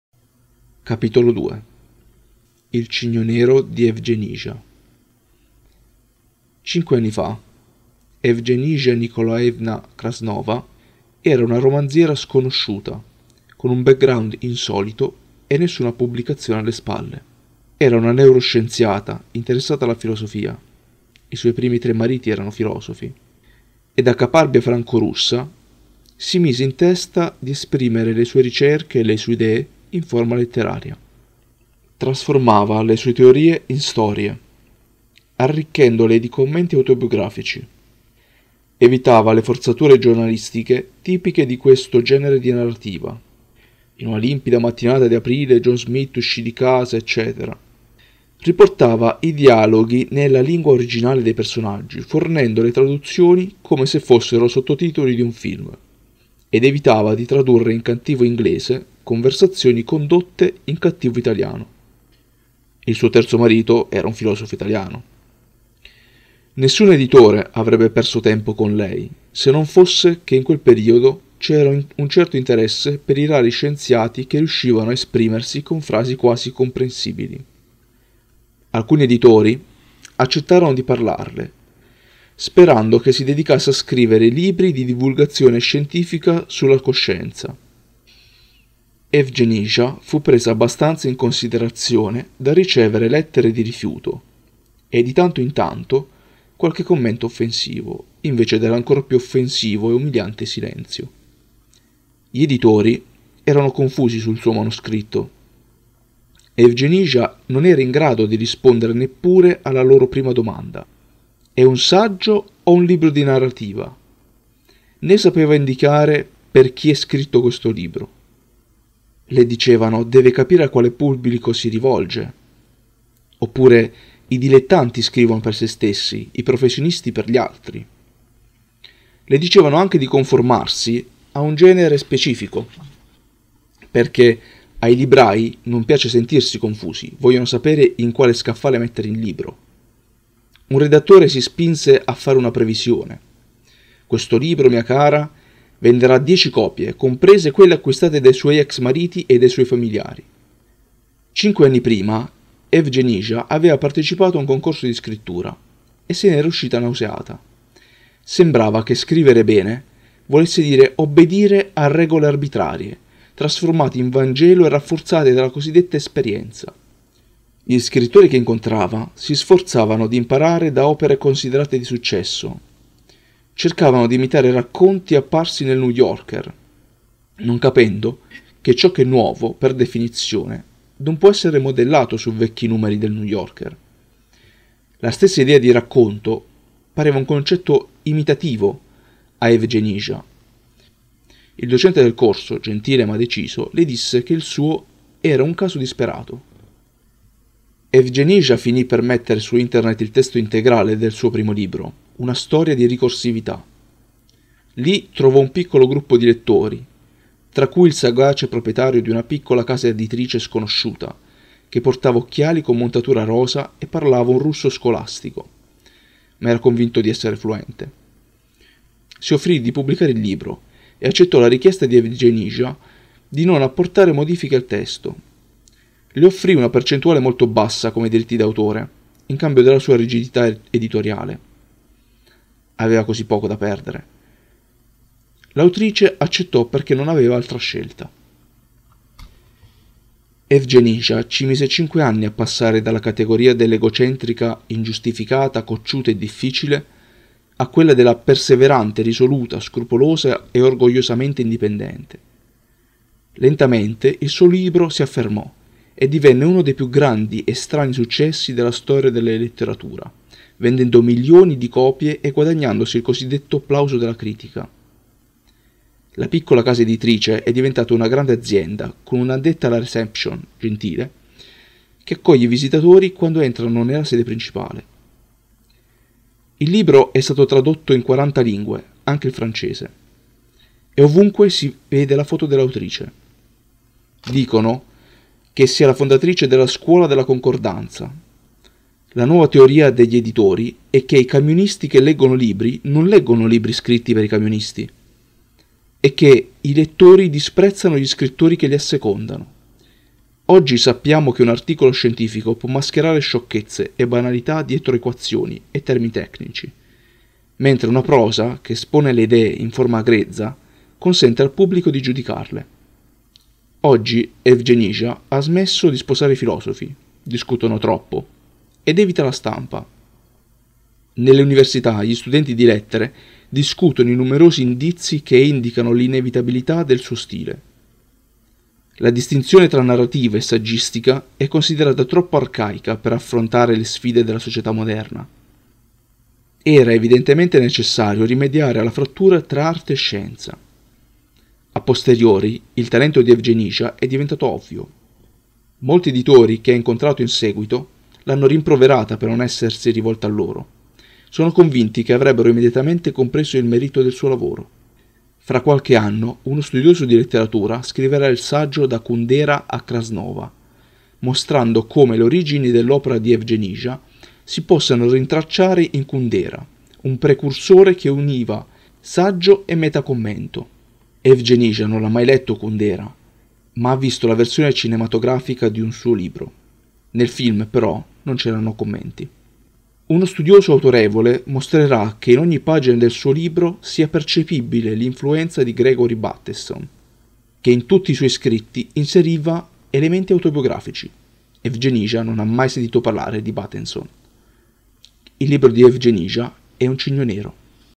Audiolibro - Il Cigno Nero - Capitolo 2
Audiolibro in Italiano de Il Cigno Nero di Nassim Nicholas Taleb, testo con note dell'autore.

00:00 IL CIGNO NERO DI EVGENIJA

Vuoi acquistare questo libro per la tua biblioteca personale?